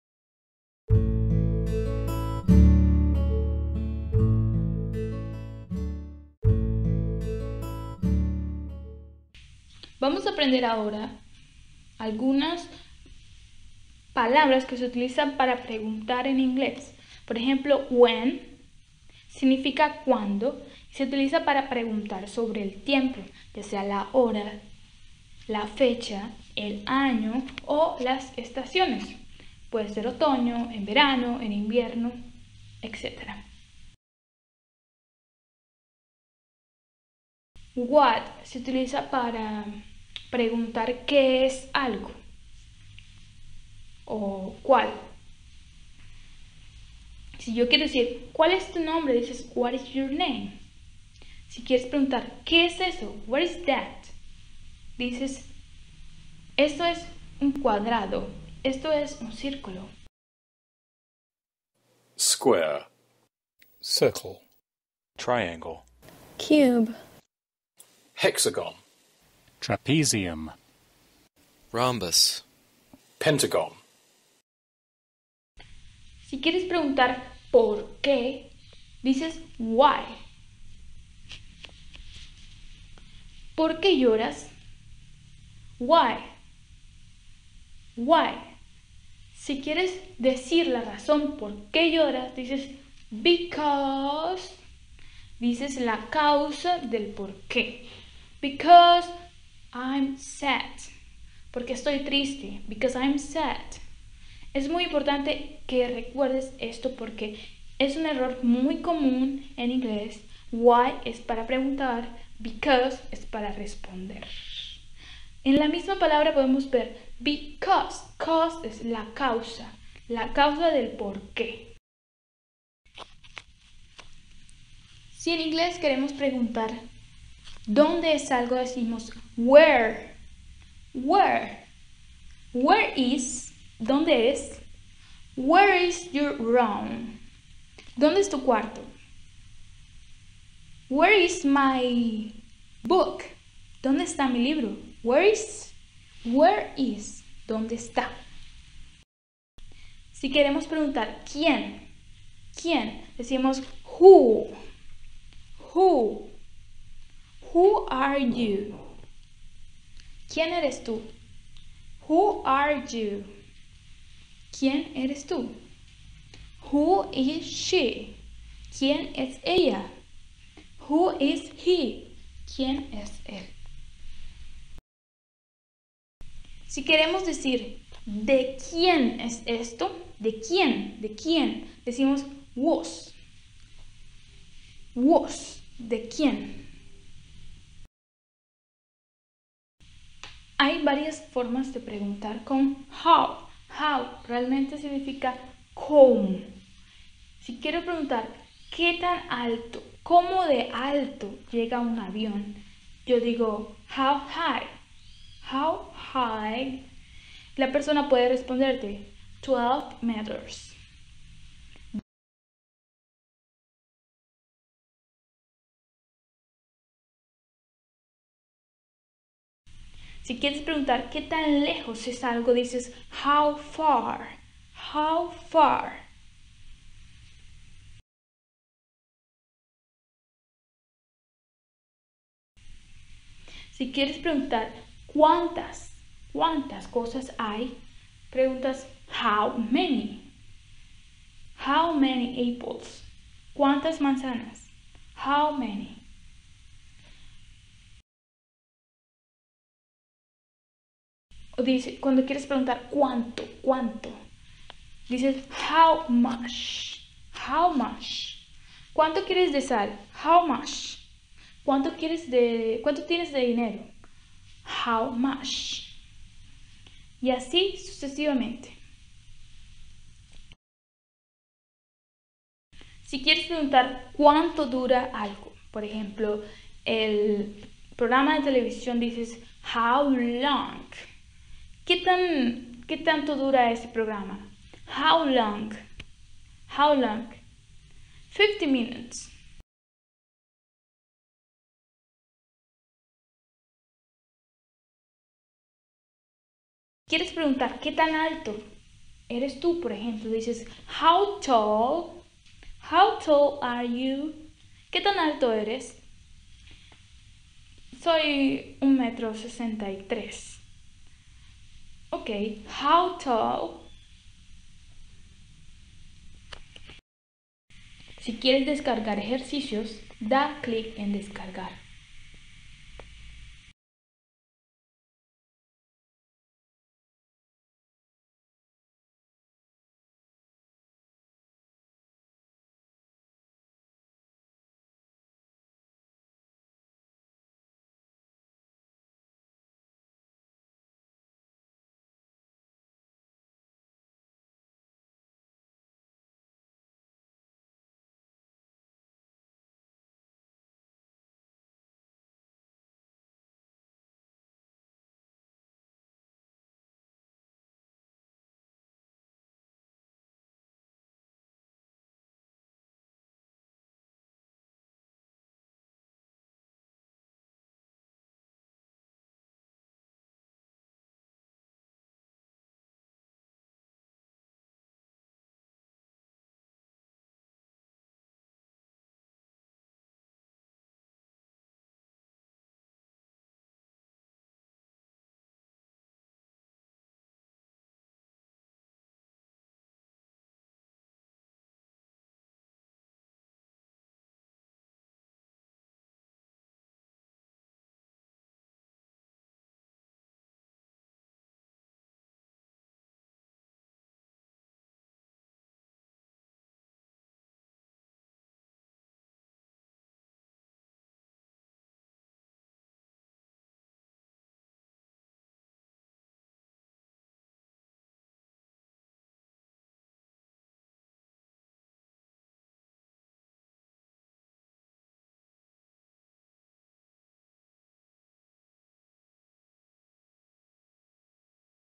Clases de Inglés básico - Wh questions / Palabras para preguntar en inglés
Clases de Inglés básico - Wh questions / Palabras para preguntar en inglés. En este sencillo video podrás aprender algunas de las palabras más comunes para preguntar en inglés. También podrás descargar ejercicios al final del video.